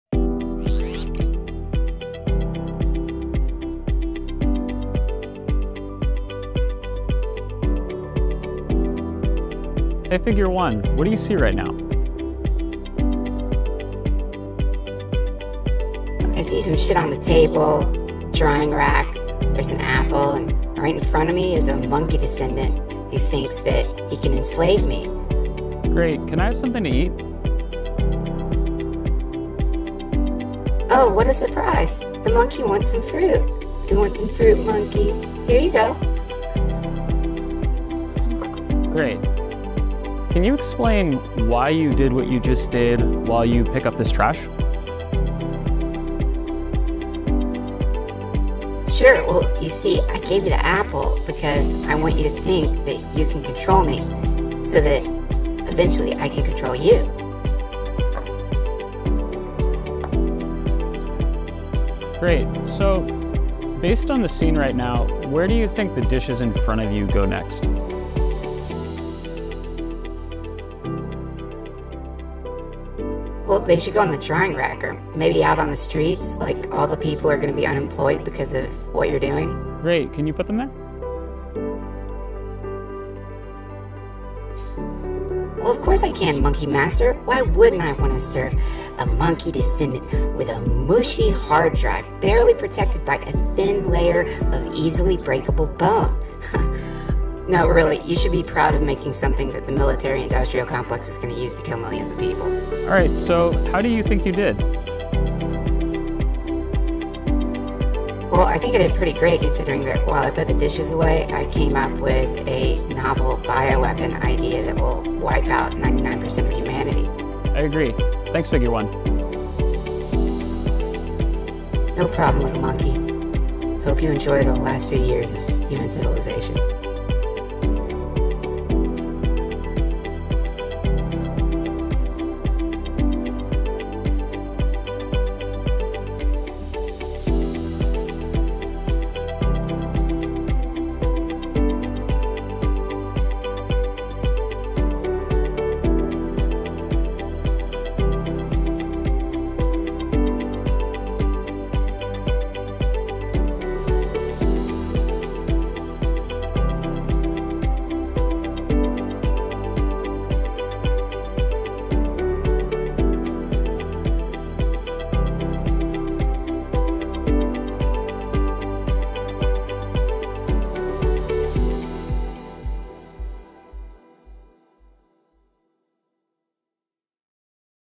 Chat GPT integrated with a robot
Chat GPT has been integrated with a humanoid robot.